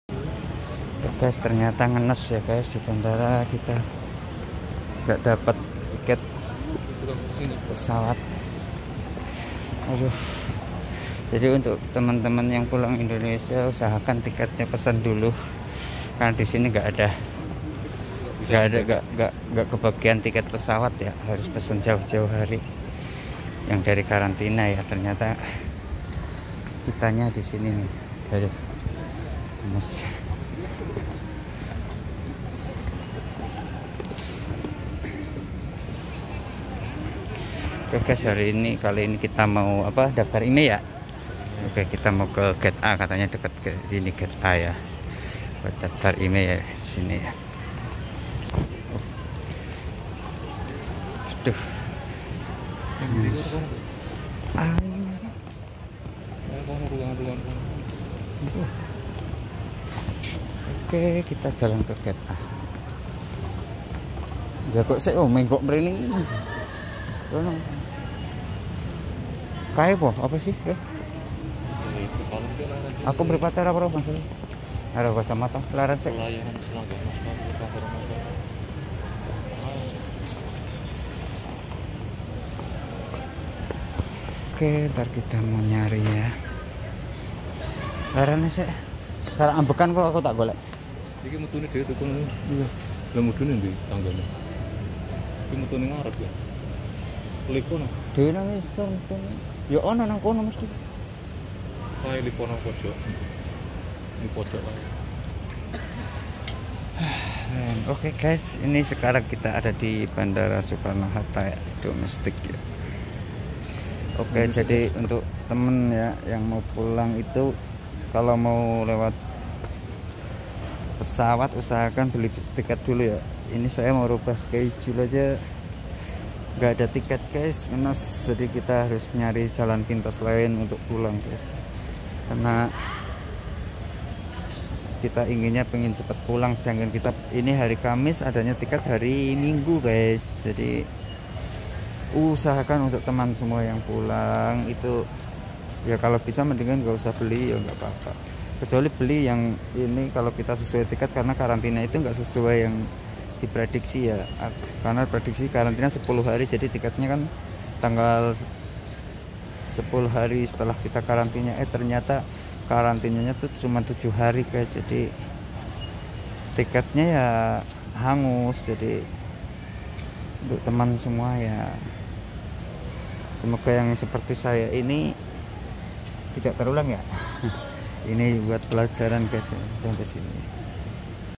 Nasib TKI Setelah Keluar Karantina ‼Bingung Pulang Ga Ada Tiket
oke guy's vedio kali ini sedikit berbagi pengalaman ya,,,tonton sampai selesai agar tahu pengalamannya ya🙏🙏

kunjungi juga IG = pino_qio22